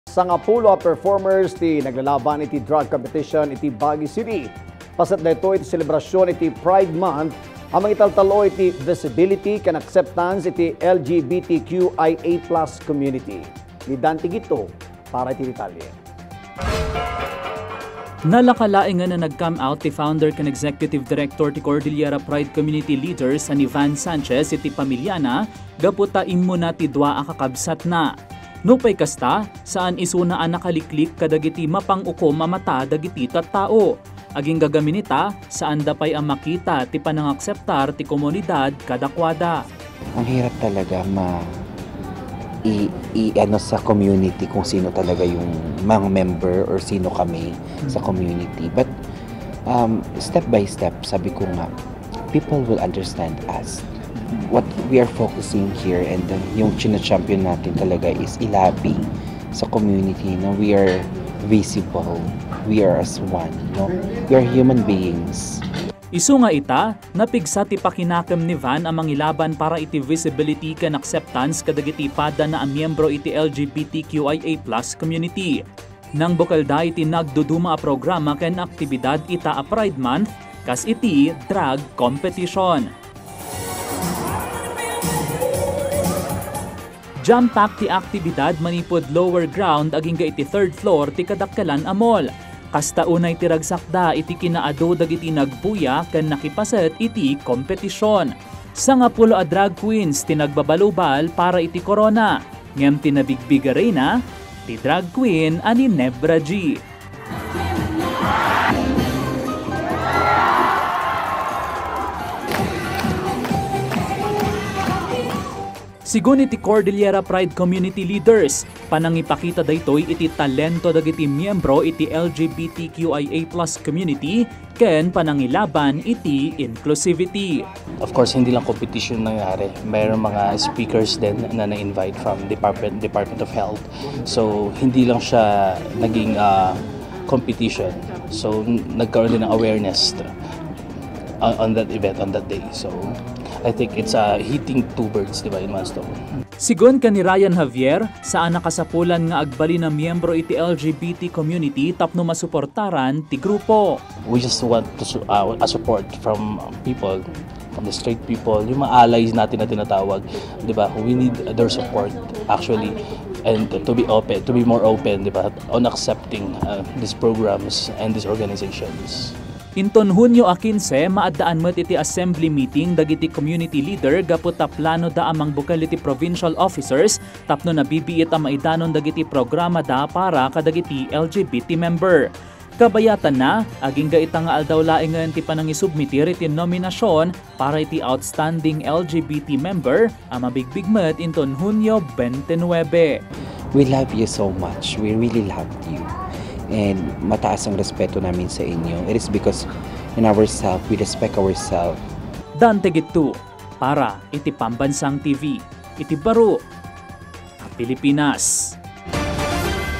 'Visibility' ken 'Acceptance', kiddaw ti LGBTQIA+ community
KangrunaanADamag | 'Visibility' ken 'Acceptance', kiddaw ti LGBTQIA+ community
• 10 Drag queens, nagbabalubal iti drag competition iti Baguio City
• CPCL, maaddaan iti Provincial officers